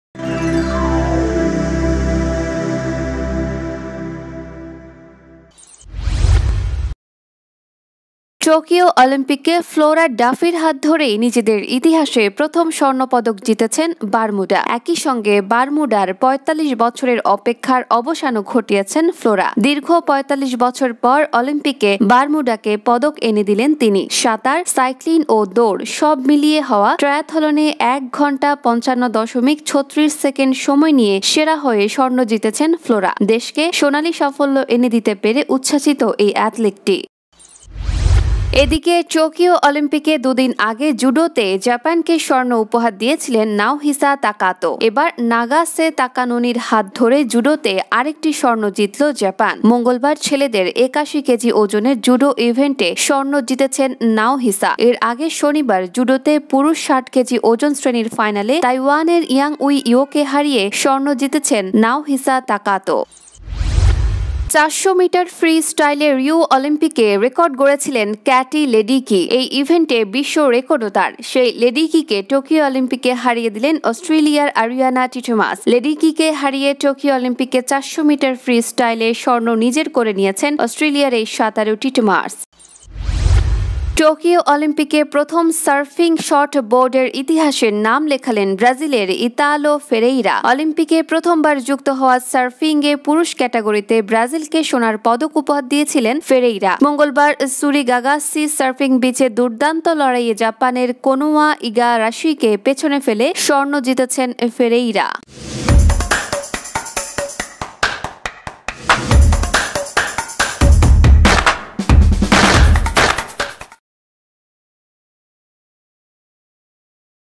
টোকিও অলিম্পিক বারমুডা, ব্রাজিলের ইতিহাস, জয়জয়কার জাপান ও অস্ট্রেলিয়ার
টোকিও অলিম্পিকে ফ্লোরা ডাফির হাত ধরে নিজেদের ইতিহাসে প্রথম স্বর্ণ পদক জিতেছে বারমুডা। একই সঙ্গে বারমুডার ৪৫ বছরের অপেক্ষার অবসানও ঘটিয়েছেন ফ্লোরা। দীর্ঘ ৪৫ বছর পর অলিম্পিকে বারমুডাকে পদক এনে দিলেন তিনি।সাঁতার, সাইক্লিং ও দৌঁড়— মিলিয়ে হওয়া ট্রায়াথলনে ১ ঘণ্টা ৫৫ দশমিক ৩৬ সেকেন্ড সময় নিয়ে সেরা হয়ে স্বর্ণ জিতেছেন ফ্লোরা। দেশকে সোনালি সাফল্য এনে দিতে পেরে উচ্ছ্বসিত এই অ্যাথলেটি। বারমুডার দ্বিতীয় অ্যাথলেট হিসেবে অলিম্পিকে পদক জিতলেন ফ্লোরা.

*This content's Copyright is reserved for National Television. Legal action will be taken against those who violate the copyright of the material.